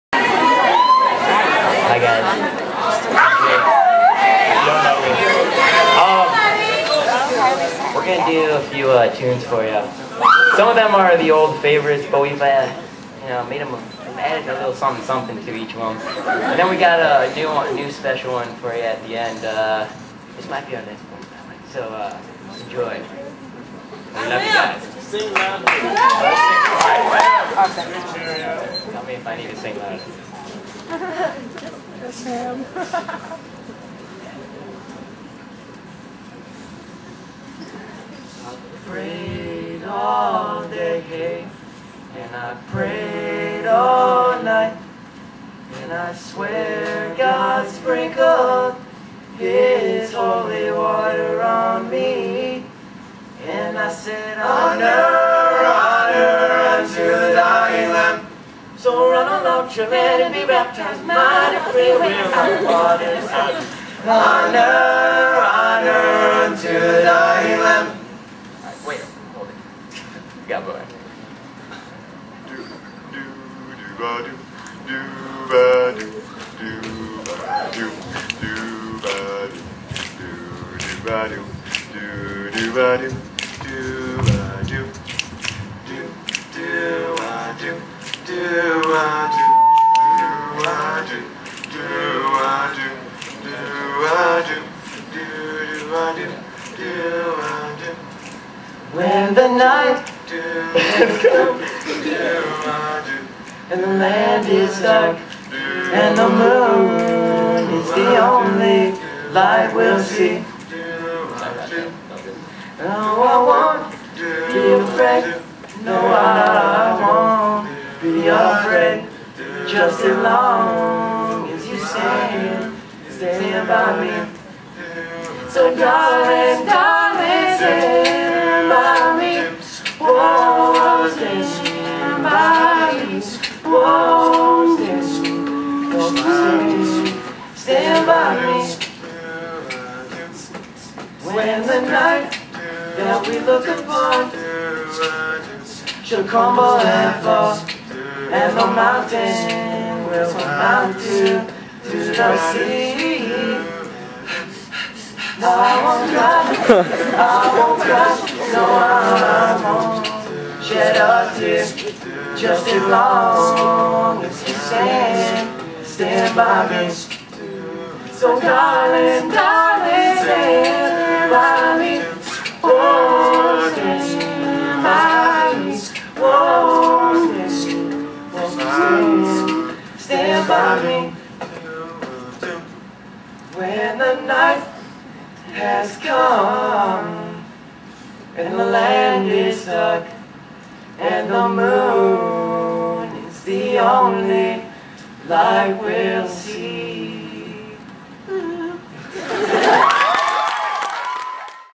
Ramah Wisconsin Choir
The Wisconsin Choir (of Camp Ramah in Wisconsin) sings at the talent show. A few bad notes, but great choice of music and cool stuff overall.